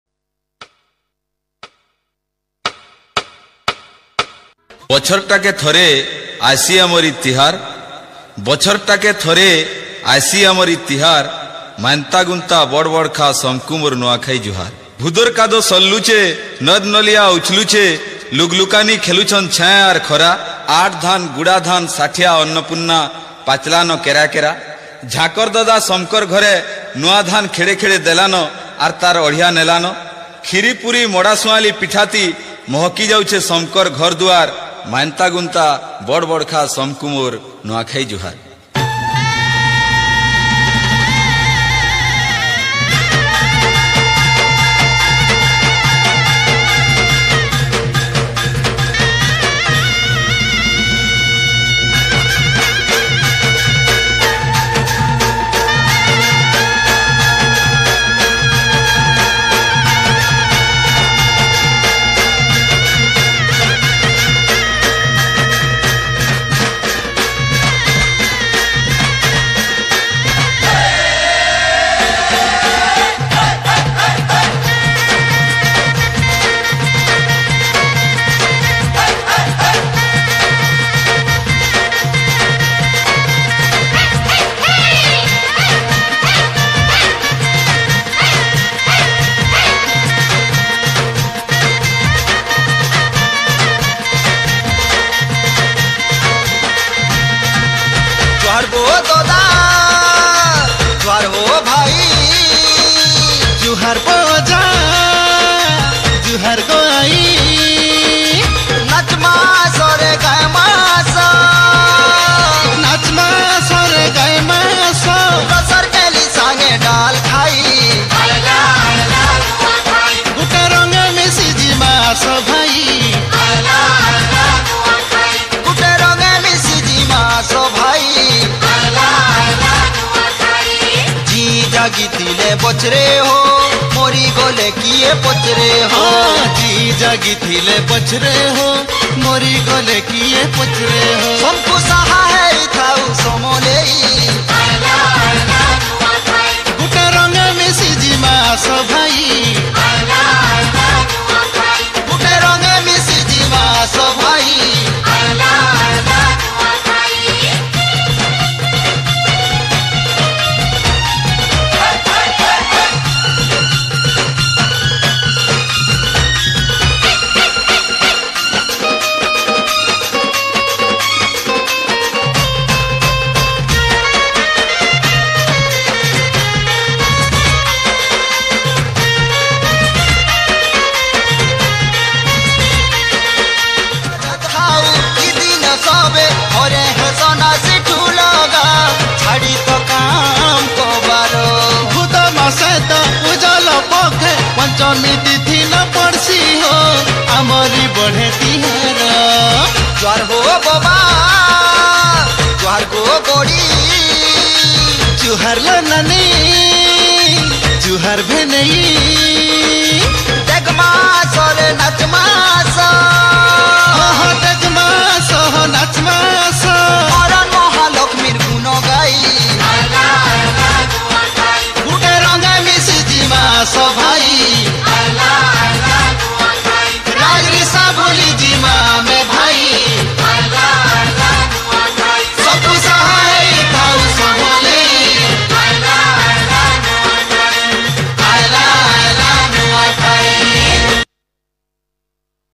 Nuakhai
Nuakhai is the famous post hervest festival of Western Odisha. In this day the new crop is being offered to the local goddess Maa samaleswari and pray her for the betterment of the people of this locality. In this day everybody wearing the new traditional dresses and enjoy the whole day with some delicious local food and also with singing and dancing.